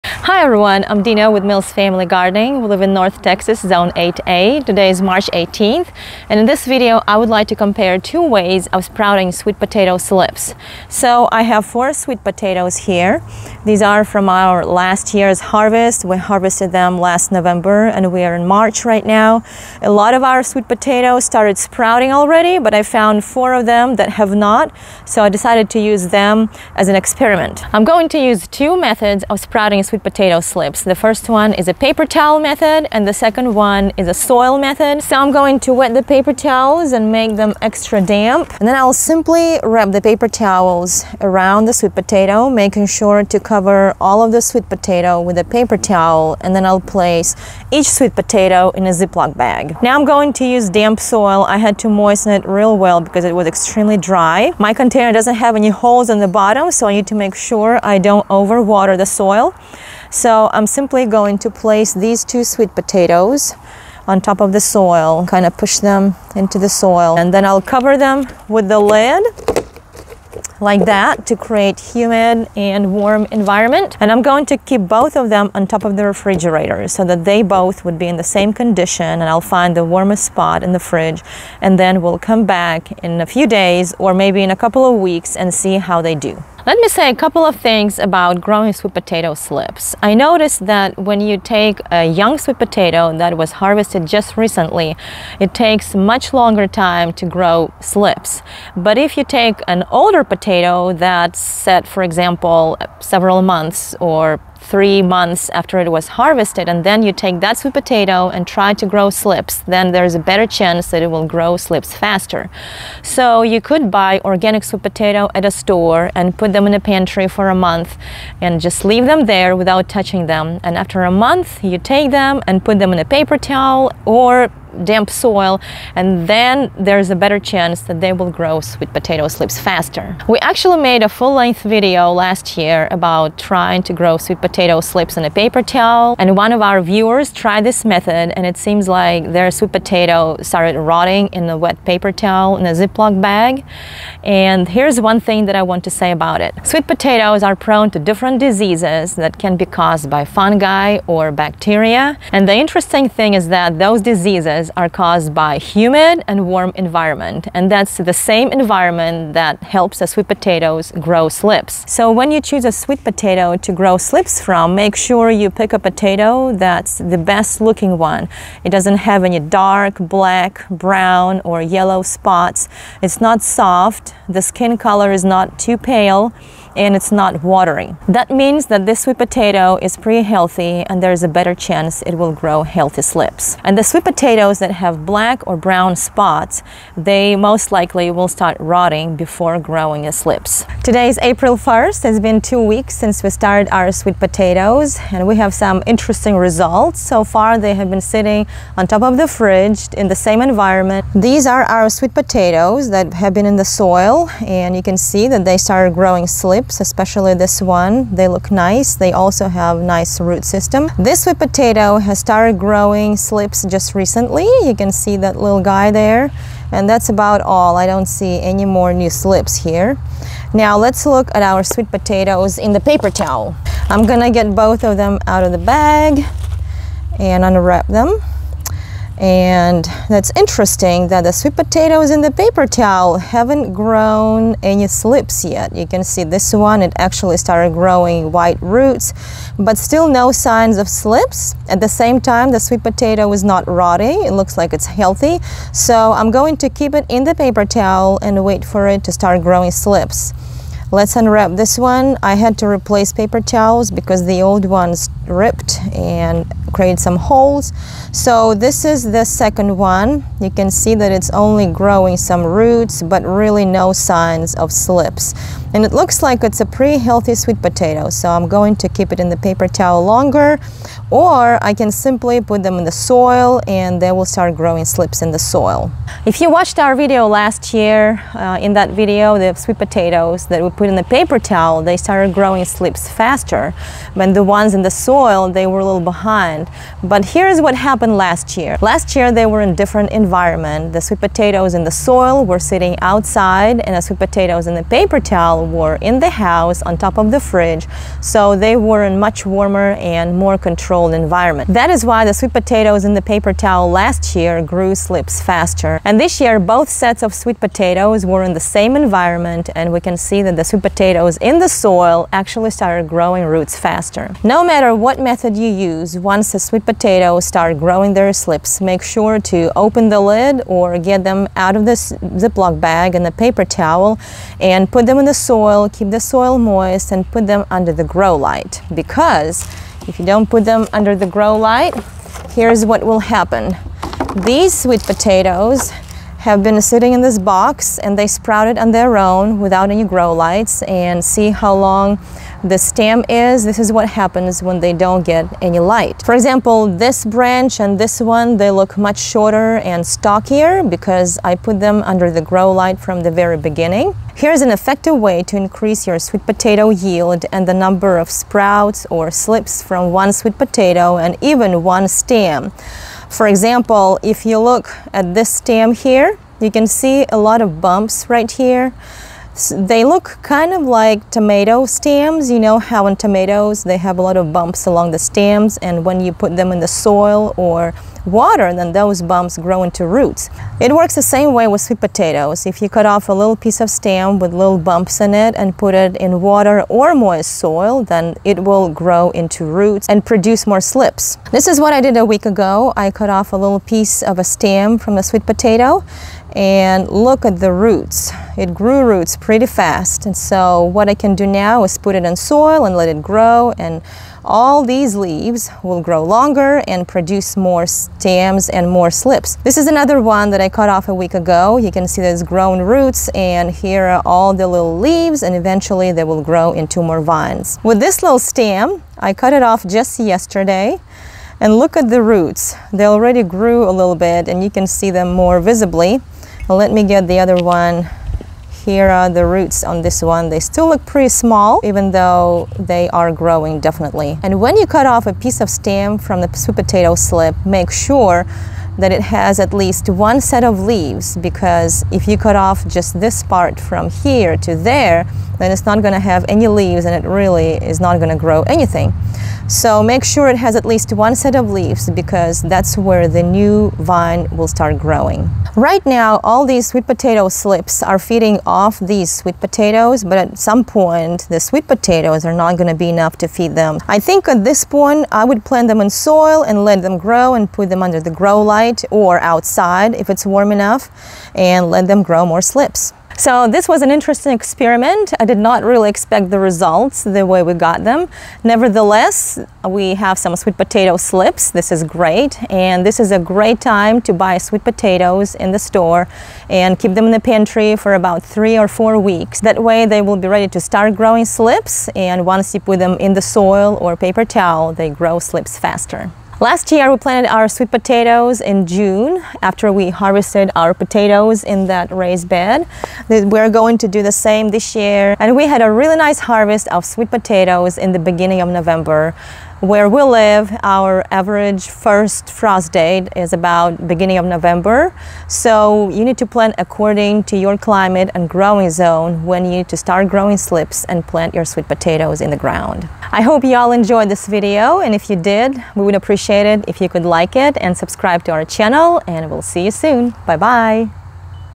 Growing SWEET POTATO SLIPS: paper towel vs. soil - How to increase yield and slips
We recently experimented with growing sweet potato slips with two different methods. Last year, we grew a nice yield of sweet potatoes in our large raised bed, and a link to that video is at the bottom. This year, we decided to try growing slips in moist soil vs. in paper towels. We previously had more success with the paper towel method, but it turns that that was because our potatoes in the moist soil were left outside in cool weather. This time, we put  them in soil in a covered container, while the potatoes wrapped in wet paper towels were zipped in plastic bags. Both were left on top of the refrigerator for a few weeks. Please check out this video to see how each did, as well as gaining a few extra tips about how to start slips. We look forward to planting them after we harvest our regular potatoes from the same raised bed.

00:00 Introduction
00:34 Sprouting sweet potatoes in paper towel
00:49 Sprouting sweet potatoes in soil
01:28 How to speed up the sprouting process
02:41 How to choose a sweet potato for growing slips
03:10 Results of sprouting slips
05:53 How to increase sweet potato yield
06:03 How to root sweet potato slips